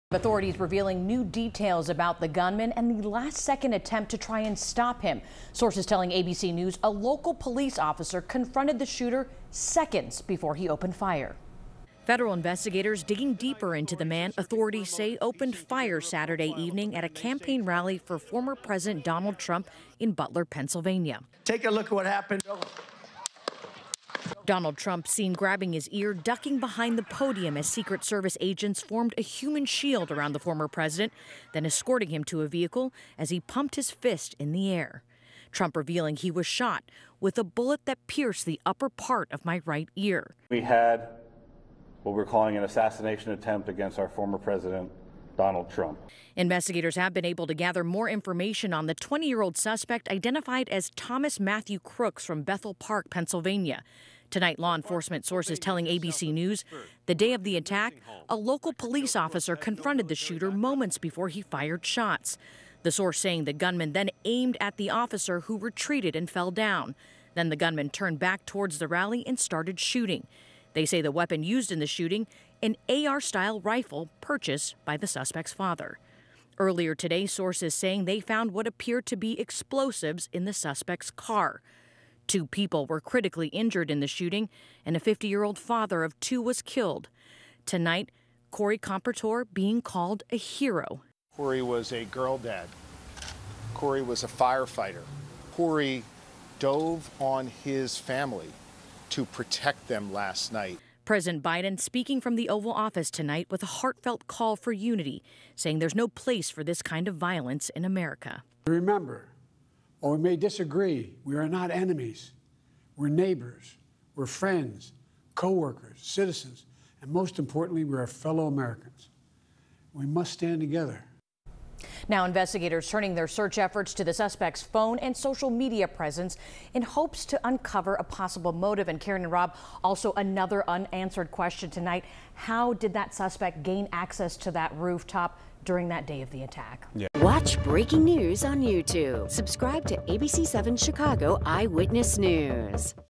Latest updates on Trump rally shooting investigation after portrait of suspected gunman emerges
The FBI identified Thomas Matthew Crooks, of Bethel Park, Pennsylvania, as the suspect in a deadly shooting at a campaign rally for Donald Trump.